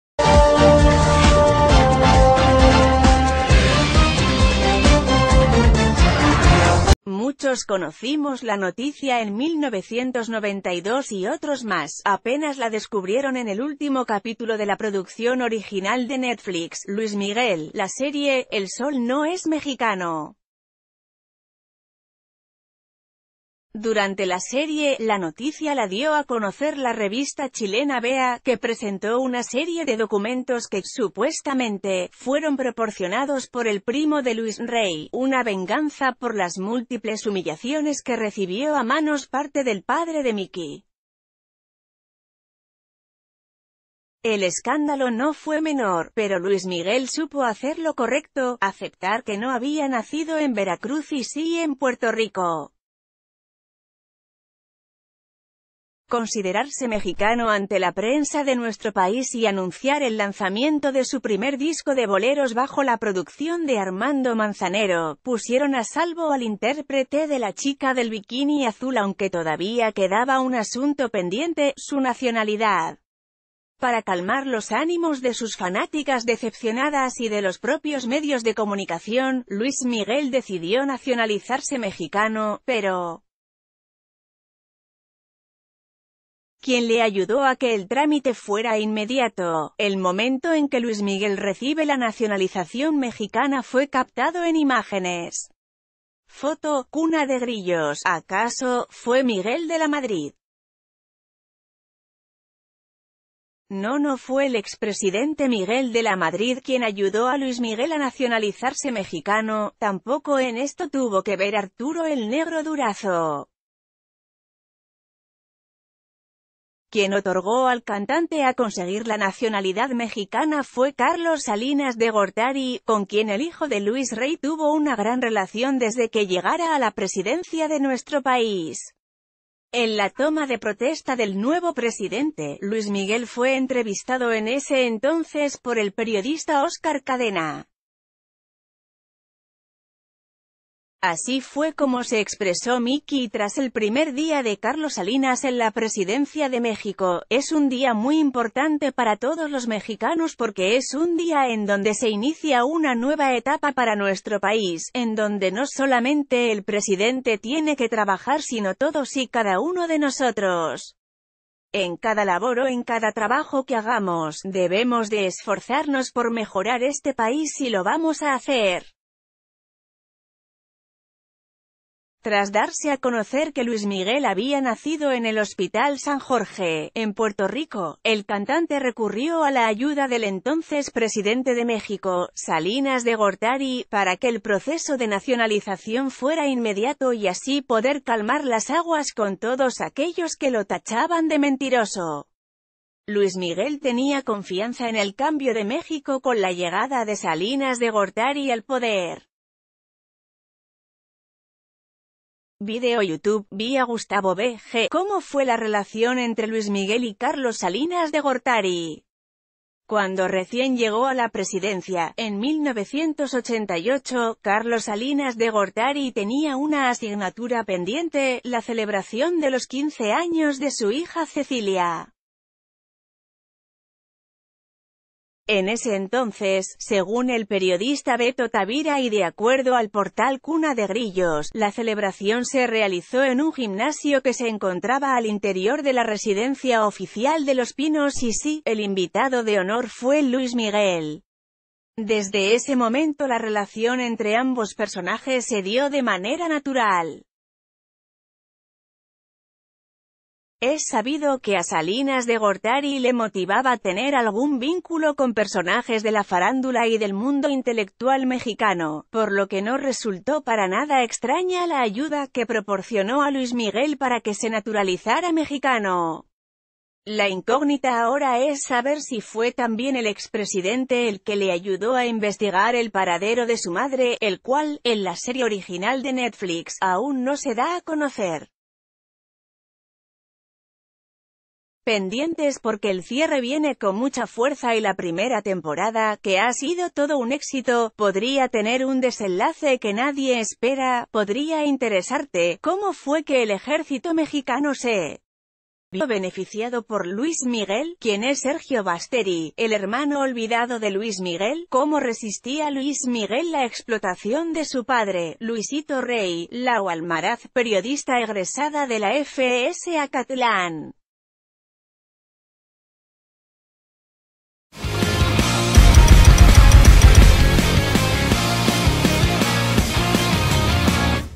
Ya sabemos que no nació en México, pero... ¿cómo obtuvo Luis Miguel la nacionalidad de nuestro país?
Muchos conocimos la noticia en 1992 y otros más, apenas la descubrieron en el último capítulo de la producción original de Netflix, Luis Miguel, La Serie: el Sol no es mexicano. Durante la serie, la noticia la dio a conocer la revista chilena Vea, que presentó una serie de documentos que, supuestame...